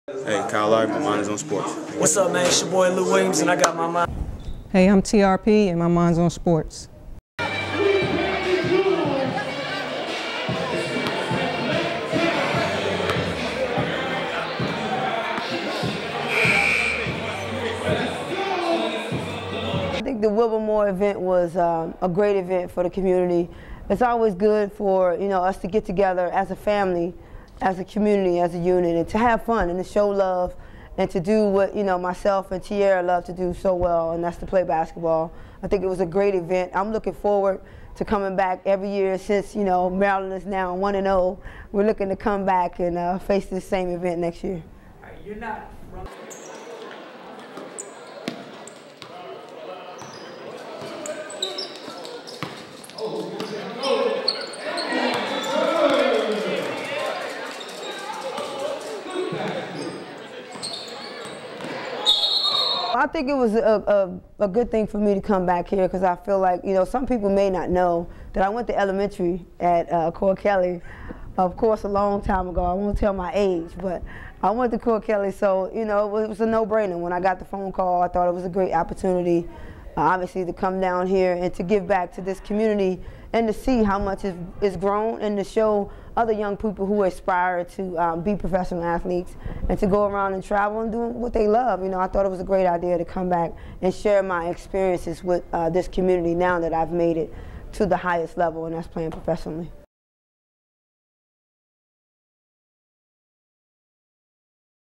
Nikki Teasley, Wilbur Moore Classic
Former WNBA player and an all time WNBA great, took some time to speak with us after the 1st Wilbur Moore Memorial Classic at Cora Kelley Rec Center in Alexandria, VA.